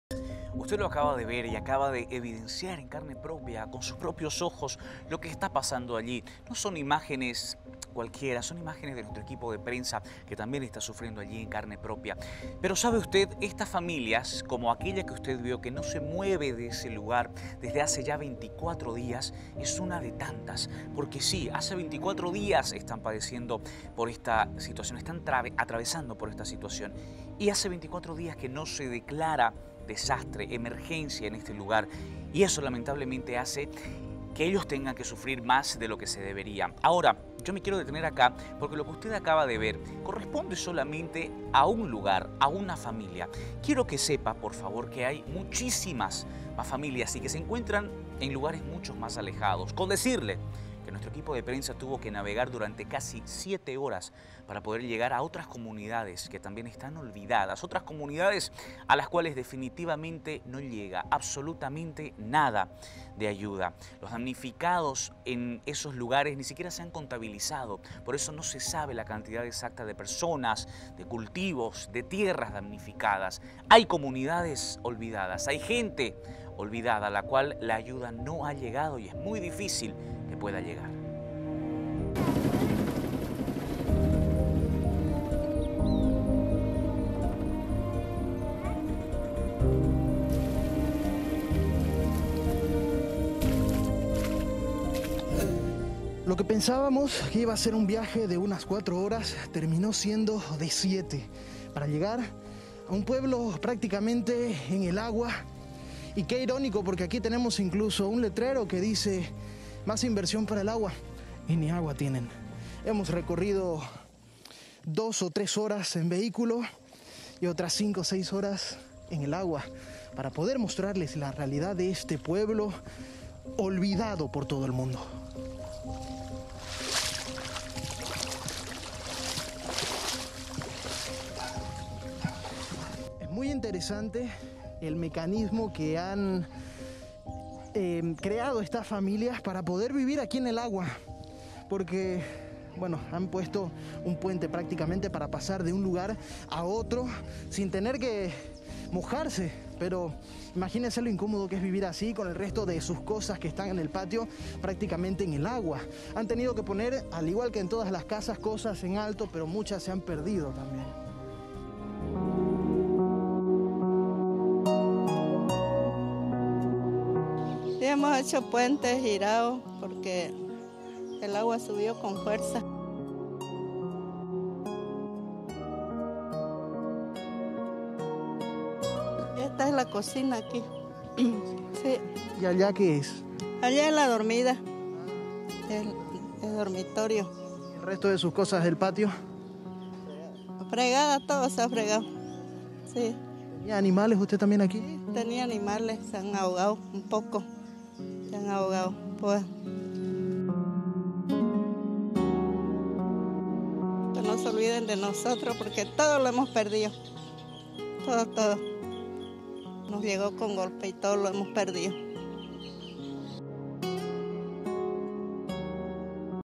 Familias enteras aisladas por el agua
Damnificados de las inundaciones improvisan en sus viviendas para poder subsistir.